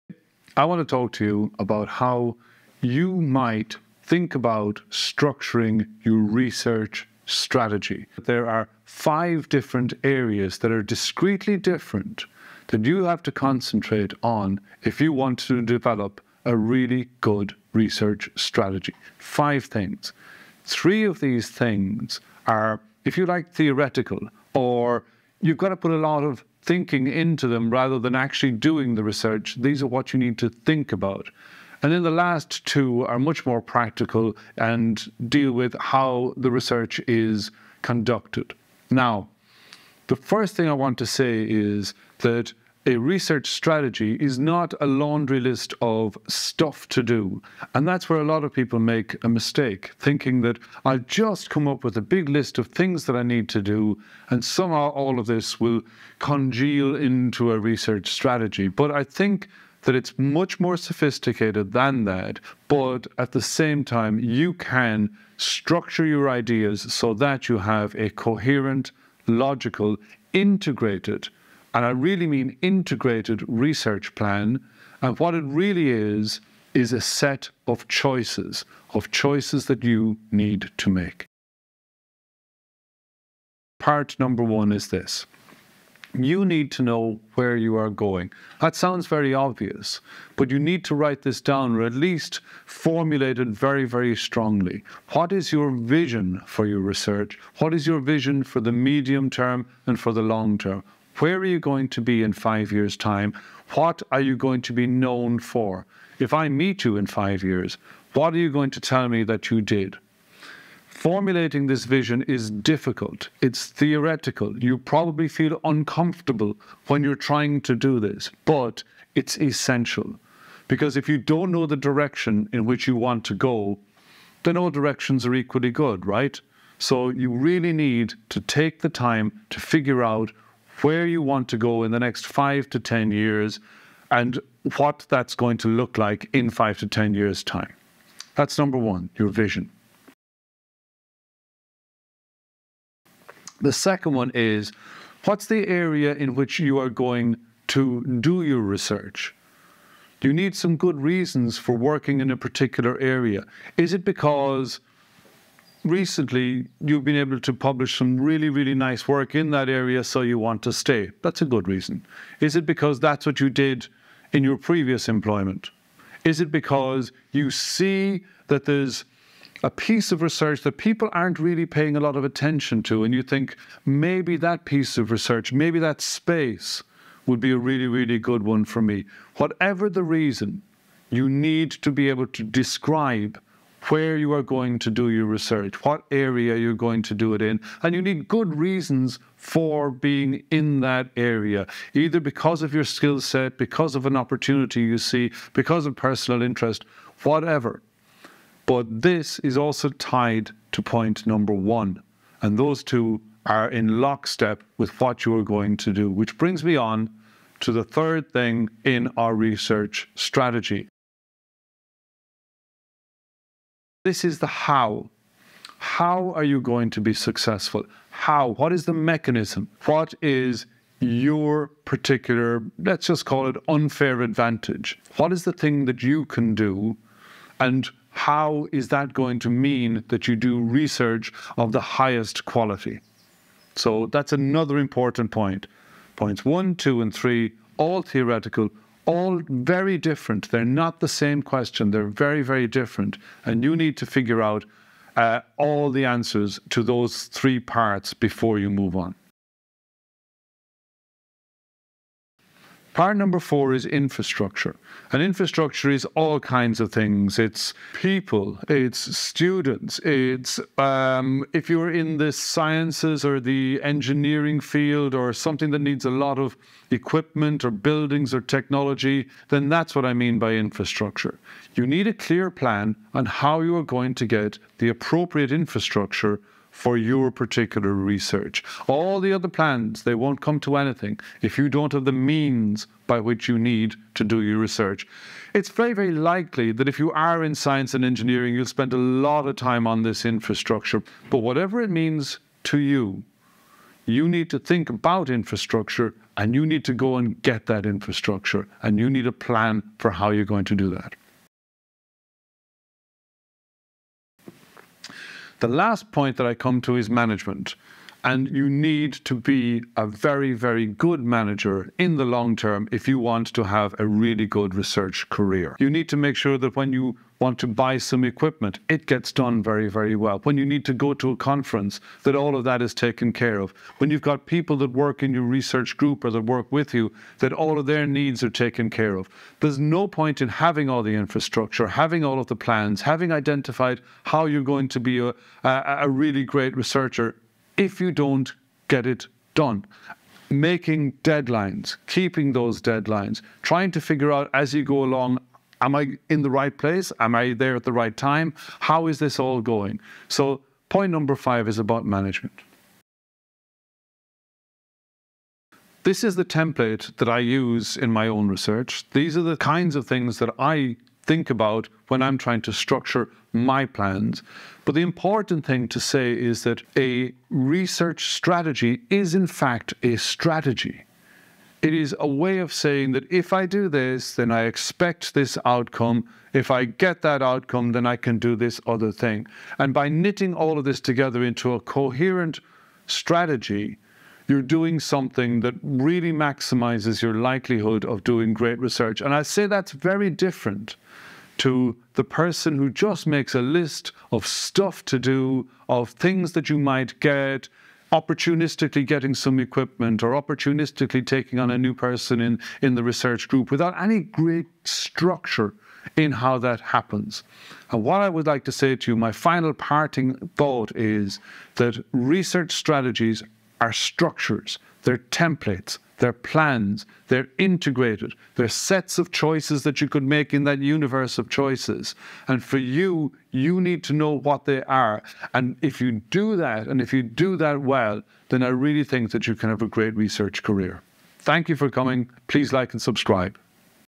Crafting Research Strategy: Five Crucial Components
🎬 NEW VIDEO IS LIVE 🎬

00:00 Introduction
01:22 Where are you going?
02:20 What Space Will You Occupy?
03:32 How Will You Succeed?
04:11 What Tools Do You Need?
05:02 How Will You Manage It All?
05:53 Wrap Up

Have you ever felt swamped planning a research project? I'm here to make things clear!  In this video where I dissect the "Five Essential Parts of a Research Strategy."

First off, we'll set our course by discussing VISION.
Next up, I'll take you through figuring out WHERE to conduct your research.
Then, we'll delve into the HOW.
We'll then take a deep-dive into the necessary INFRASTRUCTURE.
Finally, I'll introduce you to some handy MANAGEMENT TOOLS.

Whether you're a seasoned researcher or a novice, this video will be your go-to guide for crafting an effective research strategy.